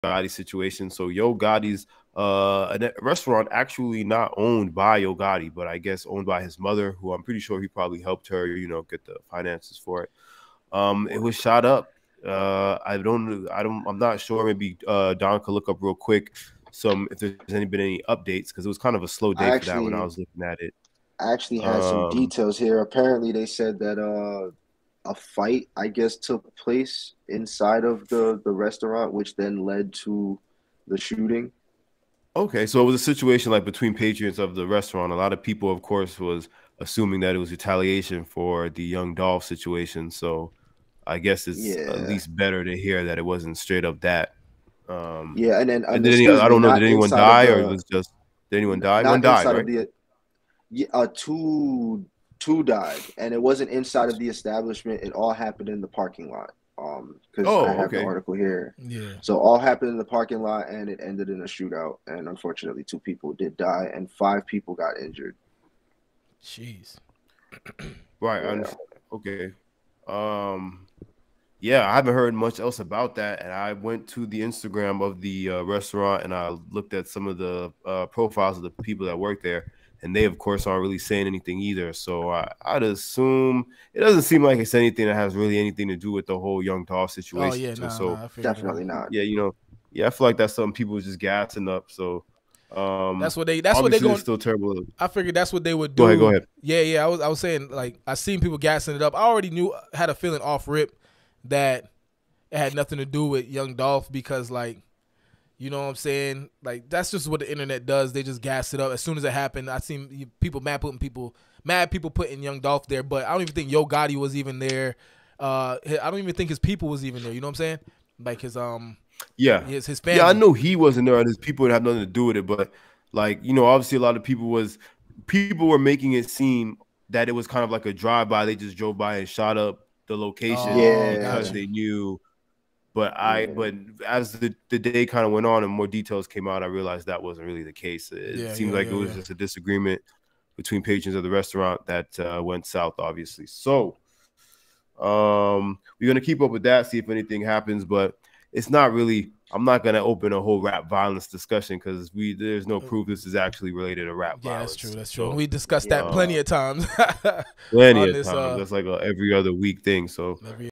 Prive Shooting: Two Dead, Five Injured at Yo Gotti’s Mom’s Restaurant
In this video, we report on the tragic shooting that took place at Prive, a Memphis restaurant owned by rapper Yo Gotti’s mother. We show you the footage of the violent brawl that started inside the restaurant and spilled out into the parking lot, where two men were killed and five others were injured. We also tell you about the suspects who have been arrested and the reaction from Yo Gotti’s family and lawyer. This is a shocking story of gun violence and its impact on the community.

$popuppodcast

Tune into the podcast on Apple Podcast! Write us a review and help us hit 100 5 star reviews.